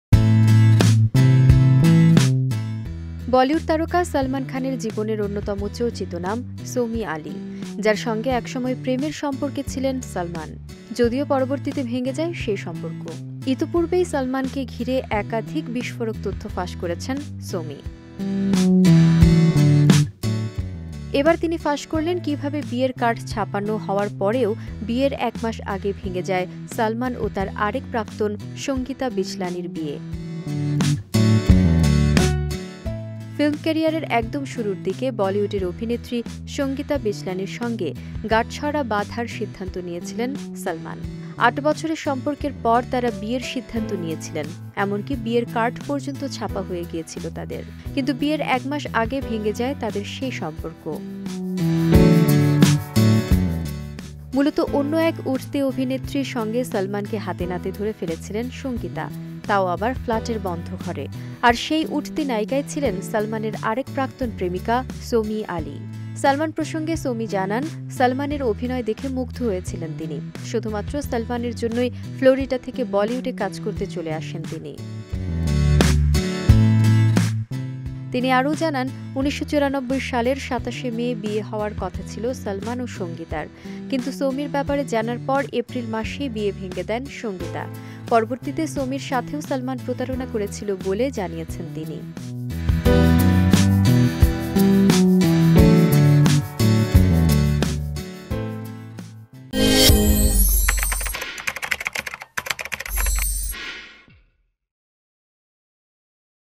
সালমানের বিয়ে নিয়ে বিষ্ফোরক তথ্য সোমি আলির! Salman । Bijoy TV
বলিউড তারকা সালমান খানের জীবনের অন্যতম চর্চিত নাম সোমি আলি। যার সঙ্গে একসময় প্রেমের সম্পর্কে ছিলেন সালমান, যদিও পরবর্তীতে ভেঙে যায় সেই সম্পর্ক। ইতোপূর্বেই সালমানকে ঘিরে একাধিক বিষ্ফোরক তথ্য ফাঁস করেছেন সোমি।
এবার তিনি ফাঁস করলেন কীভাবে বিয়ের কার্ড ছাপানো হওয়ার পরেও বিয়ের এক মাস আগে ভেঙে যায় সালমান ও তার আরেক প্রাক্তন সংগীতা বিজলানির বিয়ে।
ফিল্ম ক্যারিয়ারের একদম শুরুর দিকে বলিউডের অভিনেত্রী সংগীতা বিজলানির সঙ্গে গাঁটছড়া বাঁধার সিদ্ধান্ত নিয়েছিলেন সালমান। ৮ বছরের সম্পর্কের পর তারা বিয়ের সিদ্ধান্ত নিয়েছিলেন, এমনকি বিয়ের কার্ড পর্যন্ত ছাপা হয়ে গিয়েছিল তাদের। কিন্তু বিয়ের এক মাস আগে ভেঙে যায় তাদের সেই সম্পর্ক।
মূলত অন্য এক উঠতি অভিনেত্রীর সঙ্গে সালমানকে হাতেনাতে ধরে ফেলেছিলেন সংগীতা, তাও আবার ফ্ল্যাটের বন্ধ ঘরে। আর সেই উঠতি নায়িকাই ছিলেন সালমানের আরেক প্রাক্তন প্রেমিকা সোমি আলি।
সালমান প্রসঙ্গে সোমি জানান, সালমানের অভিনয় দেখে মুগ্ধ হয়েছিলেন তিনি। শুধুমাত্র সালমানের জন্যই ফ্লোরিডা থেকে বলিউডে কাজ করতে চলে আসেন তিনি।
তিনি আরও জানান, ১৯৯৪ সালের ২৭ মে বিয়ে হওয়ার কথা ছিল সালমান এবং সংগীতার। কিন্তু সোমির ব্যাপারে জানার পর এপ্রিল মাসেই বিয়ে ভেঙে দেন সংগীতা। পরবর্তীতে সোমির সাথেও সালমান প্রতারণা করেছিল বলে জানিয়েছেন তিনি।